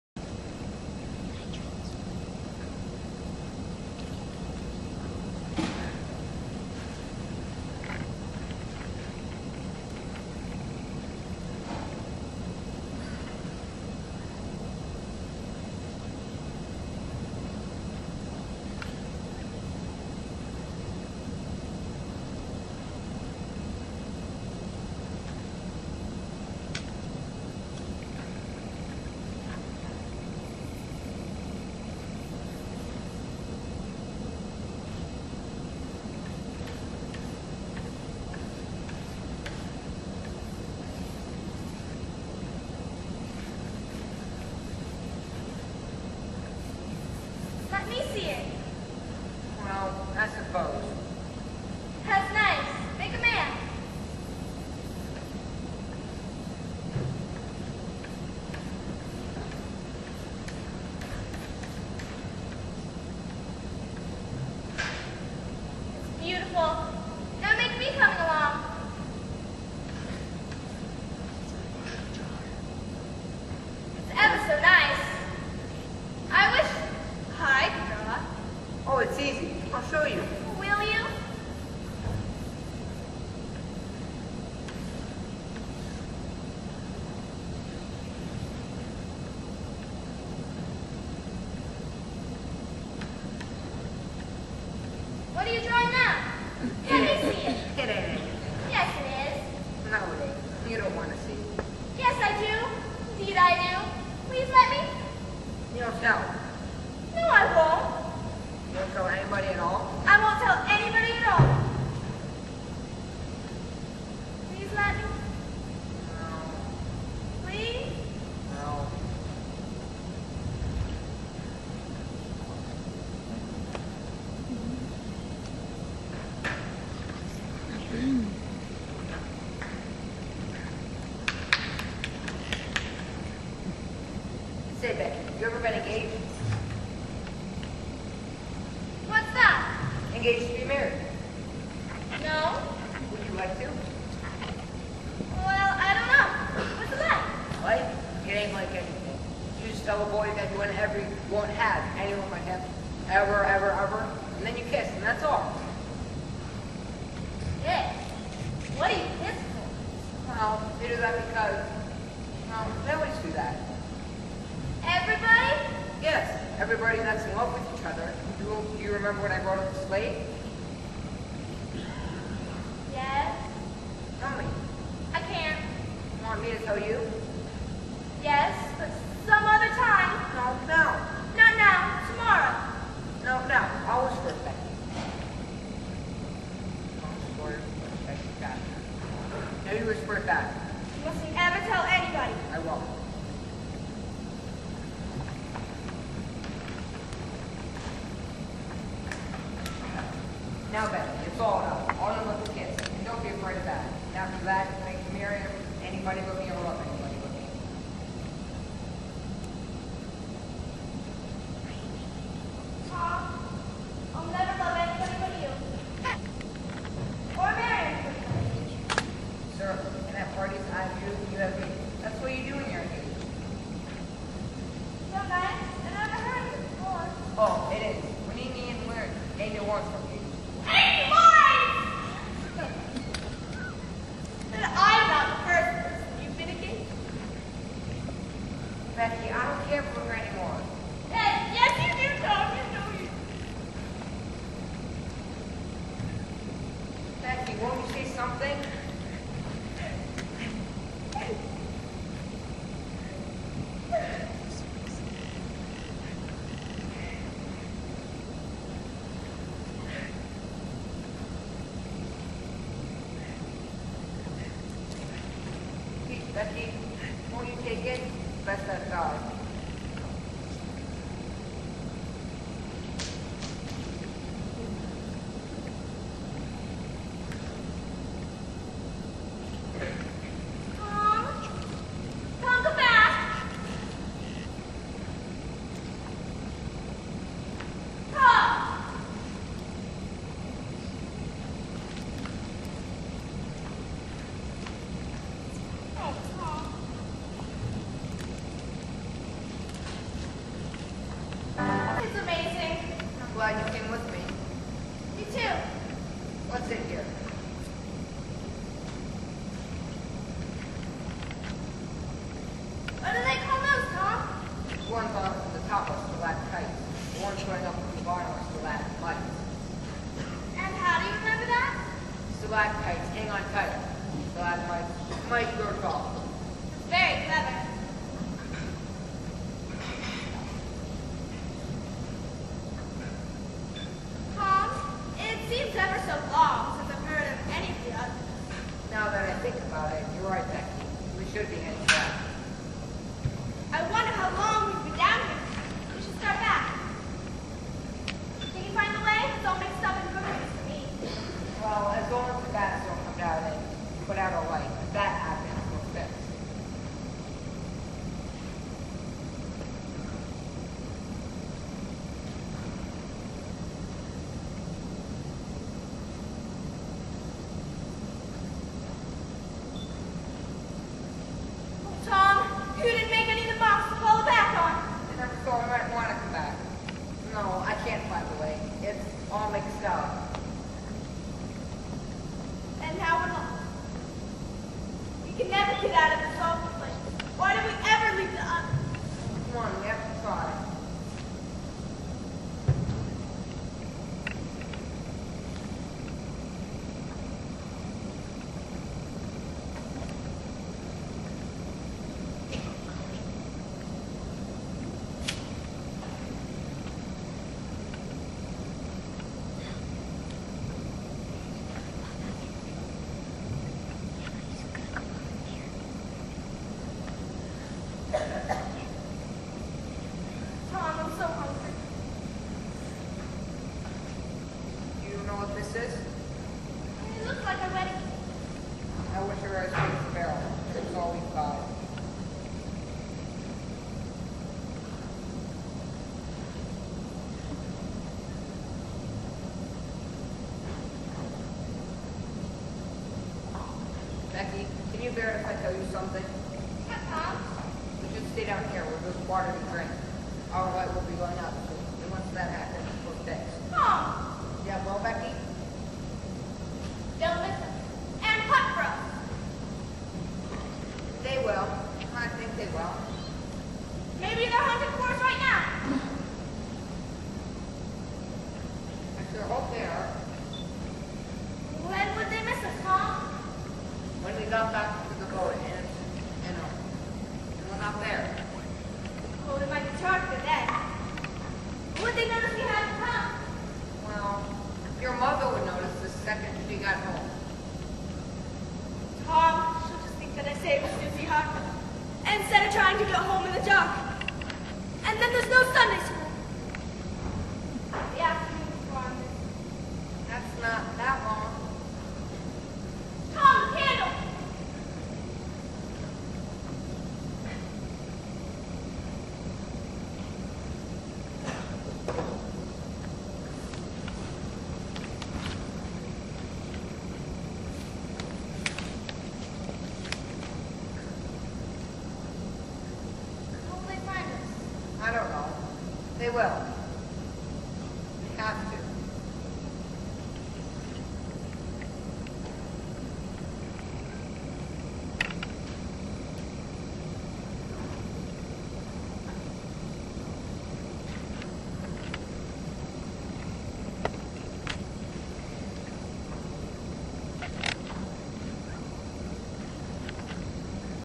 Ali In Tom Soyer Middle School Play 2012
Ali play's Becky in the fall middle school play 2012 her 7th grade yr.